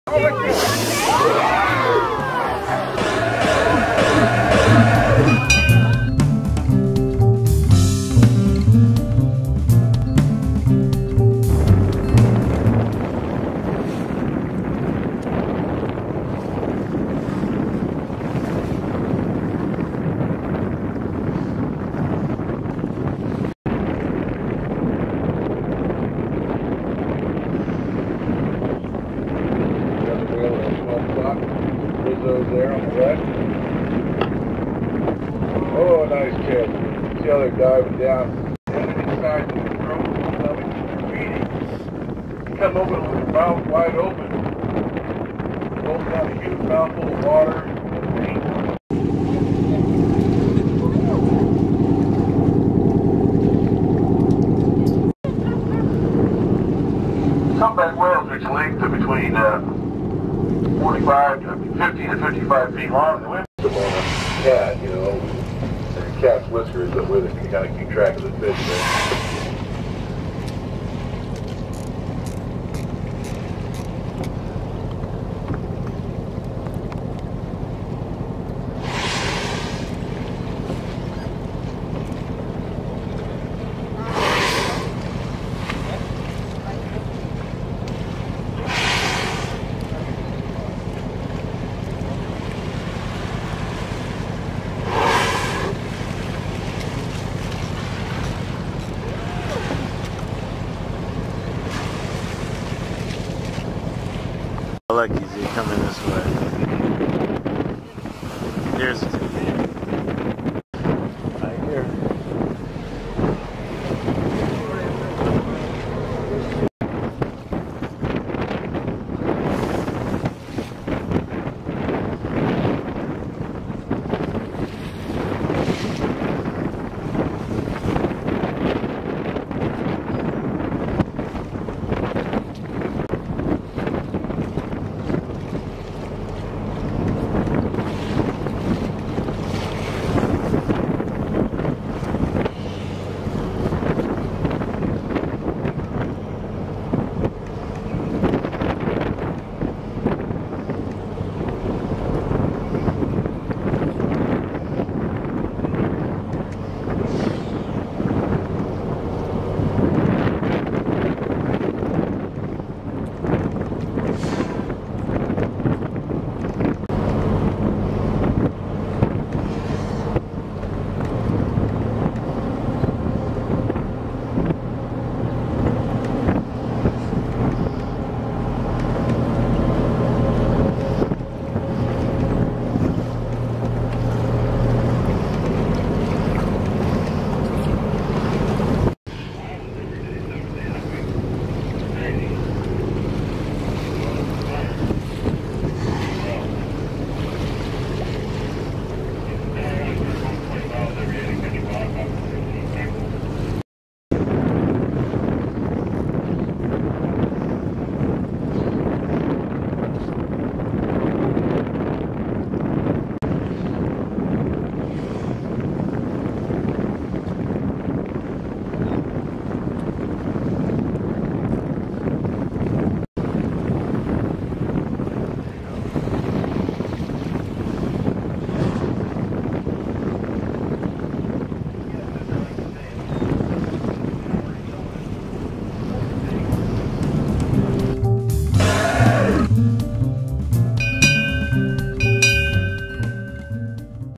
9- 22- 2009 Princess Monterey Whale Watching- sighting mag- monty
sightings from todays Pacific Explorer trips- feeding Humpbacks